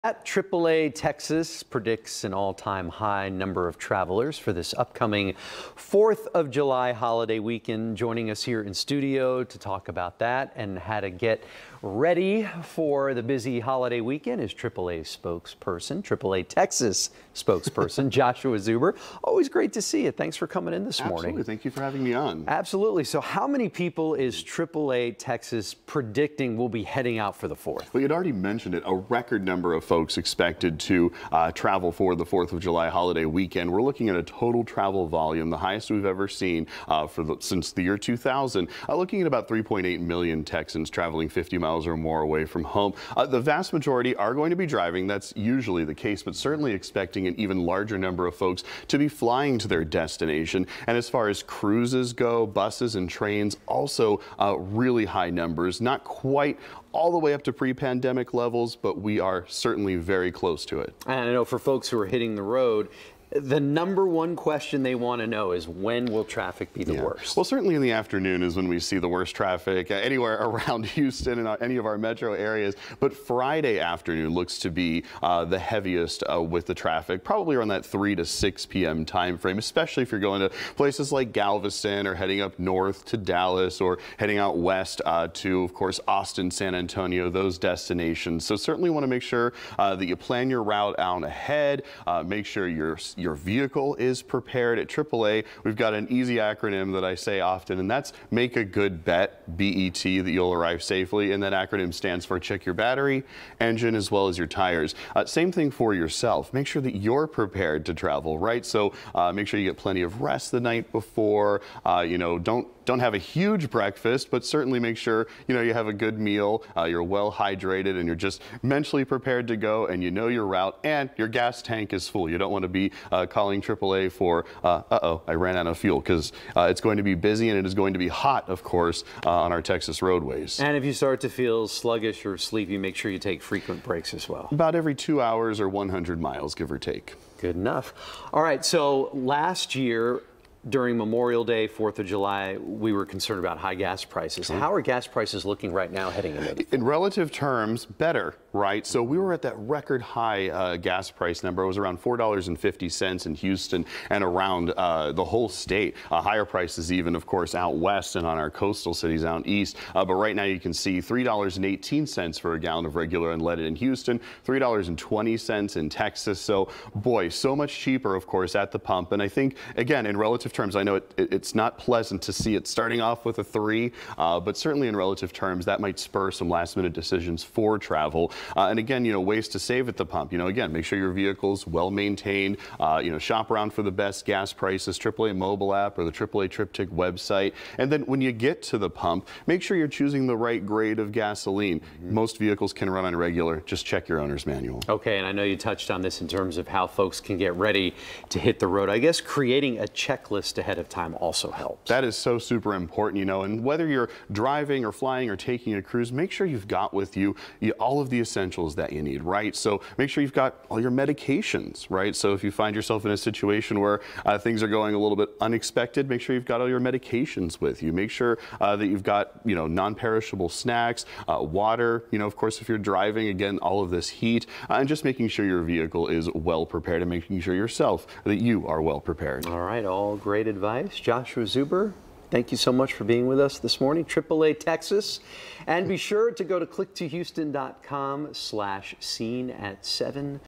AAA Texas on the busy July Fourth travel rush
AAA Texas' Joshua Zuber talks the busy travel weekend and what you need to know